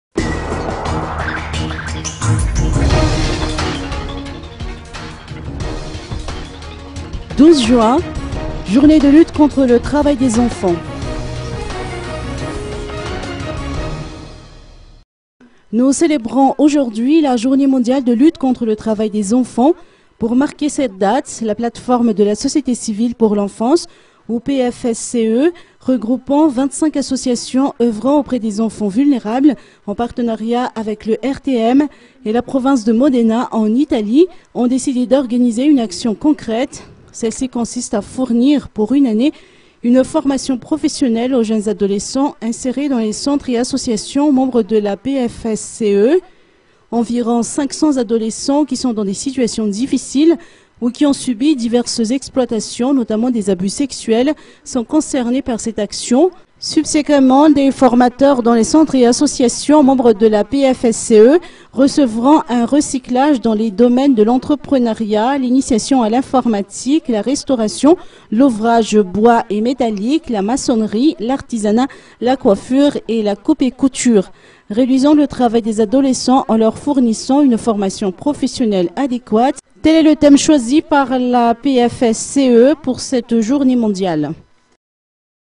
08 PFSCE
Nous célébrons aujourd'hui la Journée mondiale de lutte contre le travail des enfants. Pour marquer cette date, la Plate-Forme de la société civile pour l'enfance ou PFSCE, en partenariat avec le RTM et la province de Modena, en Italie, ont mis en oeuvre un projet.